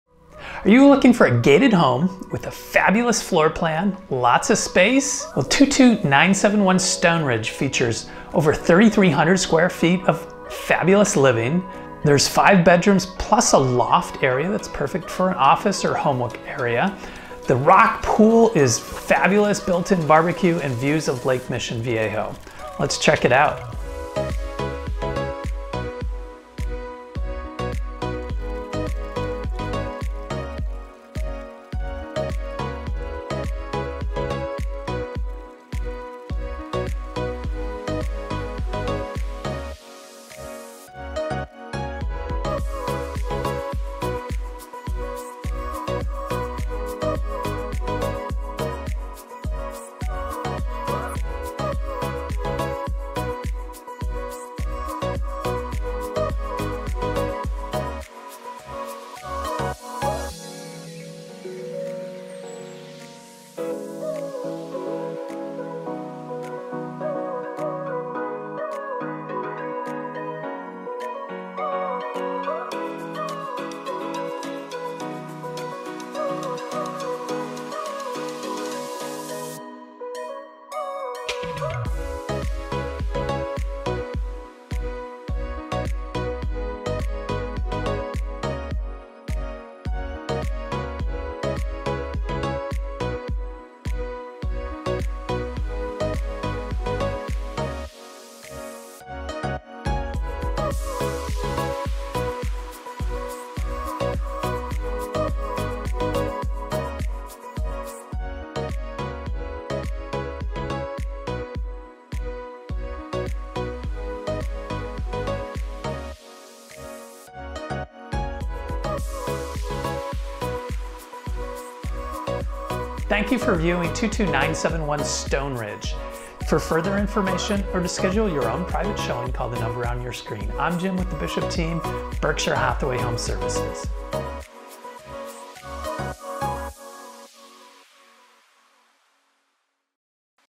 Gated Community 22971 Stoneridge Mission Viejo CA 92692
5 Bedrooms + Loft
4 Bathrooms
3,345 SF (est)
7,402 SF Lot

Welcome to the largest floor plan in the Highlands community in gated Stoneridge. This impressive home features five spacious bedrooms smartly arranged including a main floor bedroom in addition to a loft.

There is lots of natural light throughout the day. Start your day with a beautiful sunrise over the nearby hills and end with stunning sunsets out towards views of Lake Mission Viejo and the hills to the west. The enormous Kitchen/Family Room/Breakfast Nook overlook the pool and well positioned backyard. The Kitchen island wraps around smartly and offers ample space to prepare and serve your favorite meals. The upgraded Thermador 6-burner Cooktop pairs nicely with the Jenn-Air built-in refrigerator and other stainless steel appliances plus the extra-large walk-in pantry.

Enjoy fabulous hardscape upgrades throughout your spacious yard including stone accents and beautiful rock pool and spa with waterfalls and slide. The outside built-in BBQ is more appropriately described as an outdoor Kitchen with sink, side burners, storage cabinets, and refrigerator.

Attention to detail is evident throughout, starting with the custom crafted front door, to Milgard dual glazed windows, dual zone HVAC, Cat 5 wiring in all bedrooms, and Monier Lifetile concrete tile roof.

Stoneridge is a small guard-gated enclave at the top of Mission Viejo, close to the Lake, Flo Jo Park, and all the great amenities Mission Viejo has to offer. You’ll have lake privileges that include the private beach, summer concerts, movie nights, 4th of July extravaganza, boat rentals, stocked fishing and more. Close to world famous beaches, shopping, transportation, restaurants and more. Come discover the Stoneridge lifestyle today!